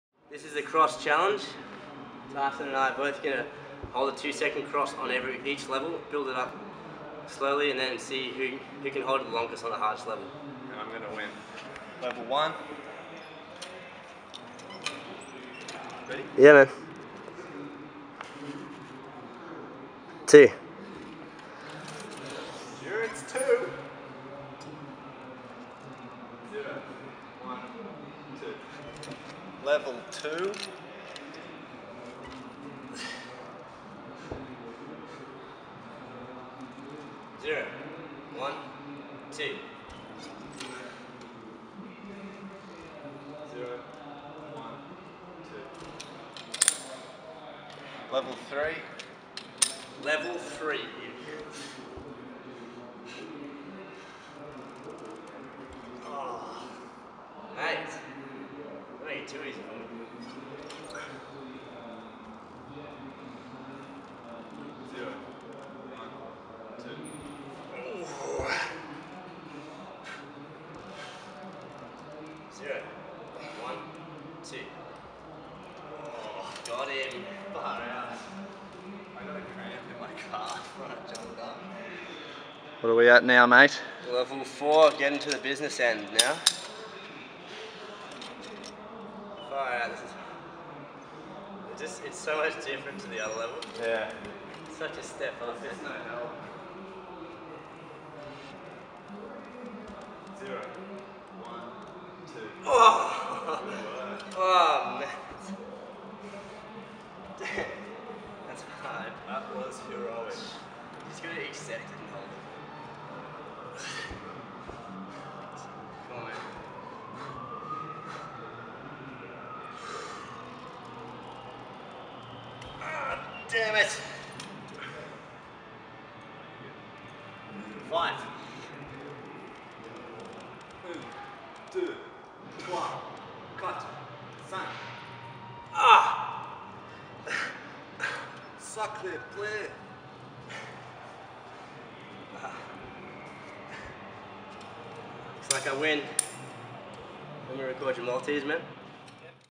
Iron Cross Strength Challenge
A fun way we challenge ourselves to hold longer crosses.